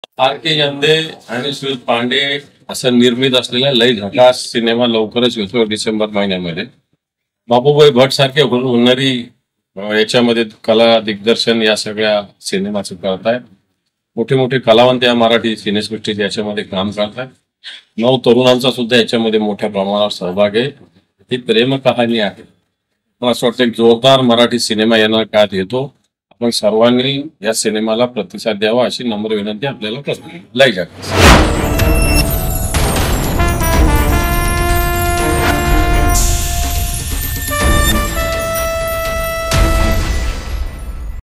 Marathi movie|Ambadas danve | Lai jhakaas |Mukesh Bhatt|Manisha Singh
आपल्या भेटीसाठी लवकरच सिनेमागृहात येणाऱ्या "लई झकास" सिनेमासाठी विरोधी पक्षनेते, महाराष्ट्र विधान परिषद अंबादास दानवे यांनी भरभरून शुभेच्छांचा वर्षाव केला आहे.यशाच्या मार्गावर आरूढ असलेला हा सिनेमा नक्कीच काहीतरी विक्रम करेल..
Lai jhakaas is releasing on December 15th in cinemas near you.

Production House - RKY Films , Pande patil
Produced by - Kishor Yande, R.N.Pande Patil
Directed By - Babu bhatt
Choreographed by - Kiran Giri
Star Cast - Mukesh Bhatt, Manisha Singh, Sanjay Narvekar, Nisha Parulekar, Nagesh Bhosle, Uday Tikekar, Sunil Tawde, Sanjay Kulkarni,
Nishigandha Wad ,Manoj Joshi & Sachin Gawli.

13m